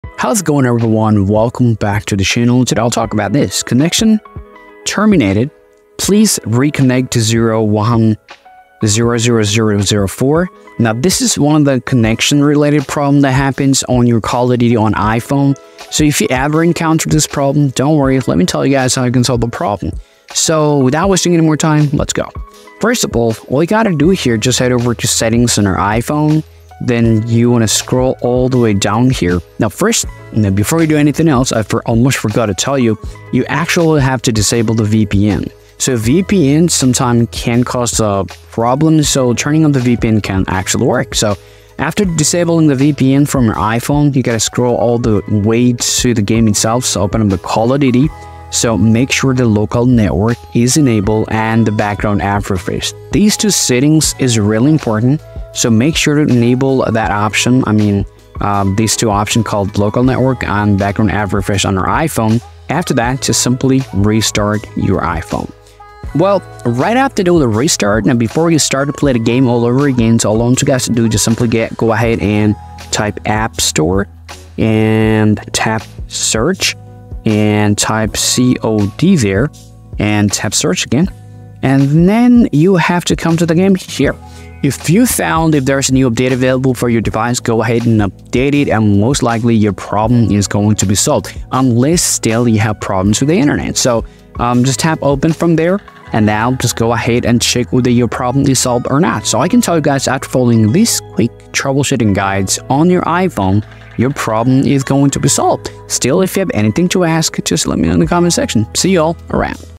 how to fix the Connection Terminated error in Call of Duty Mobile on iPhone
Facing connection terminated Call of Duty Mobile? No worries, I have come up with a solution video tutorial on how to fix the Call of Duty Mobile connection terminated error. Watch the video guide and discover how to solve the Call of Duty connection terminated.

In this quick video tutorial, I'm gonna show you the right method to solve connection errors on Call of Duty Mobile. So, watch the video and follow the instructions step by step carefully. Learn the best process of solving COD Mobile connection Terminated error.

00:01- Video intro
00:22- The Solutions
01:39- Solution ending